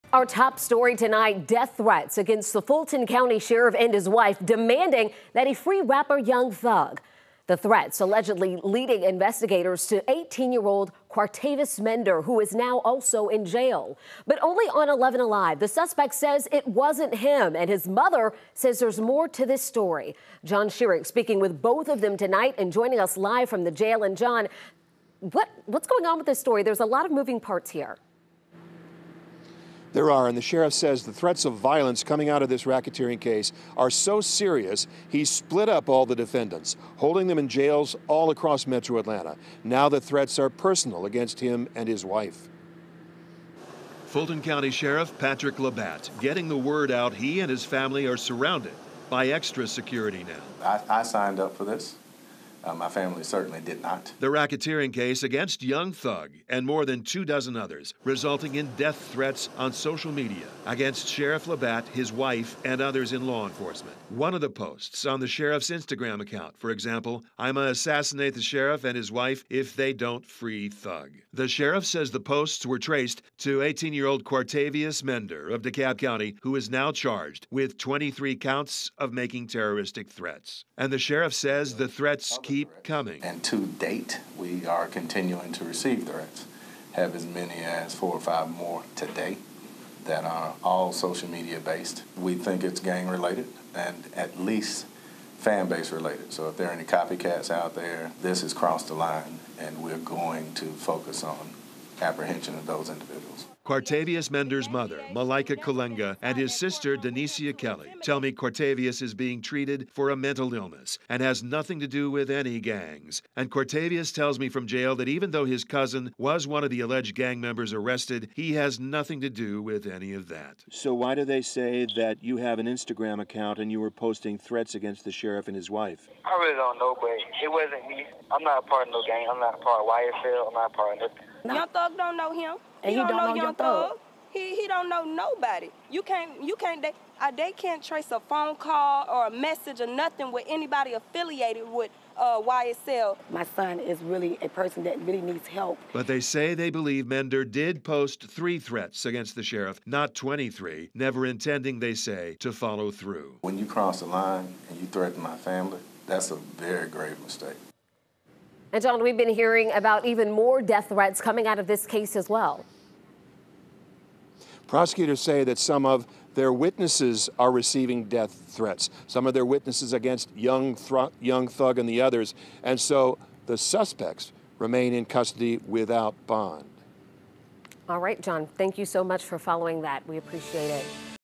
Mother of teen arrested for making social media threats over YSL jailings speaks out
The threats were said to be made over social media.